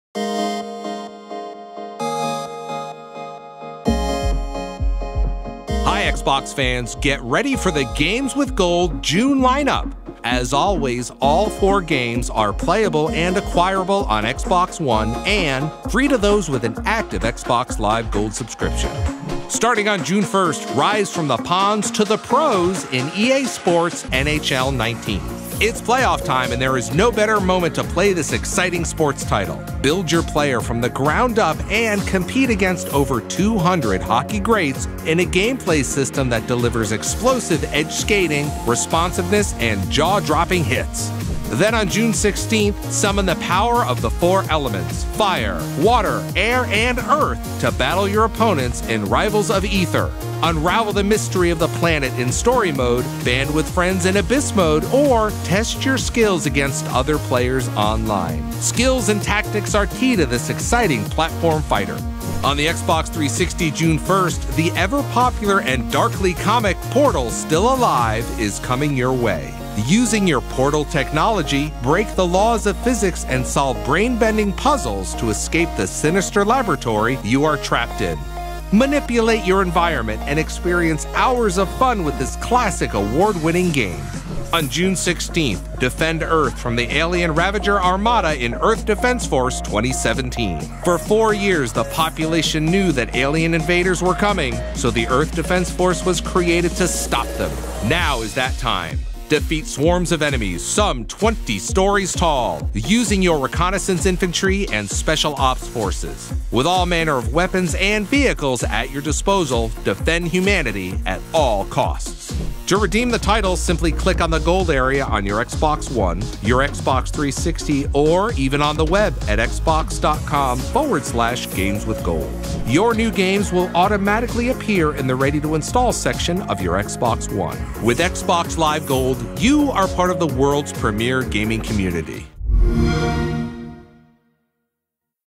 Juegos gratis de Xbox Live Gold para junio 2019
Para Xbox1: EA SPORTS NHL® 19 y Rivals of Aether. Para Xbox 360 Portal: Still Alive y Earth Defense Force 2017 son los juegos gratuitos para los miembros de Xbox Live Gold para junio 2019.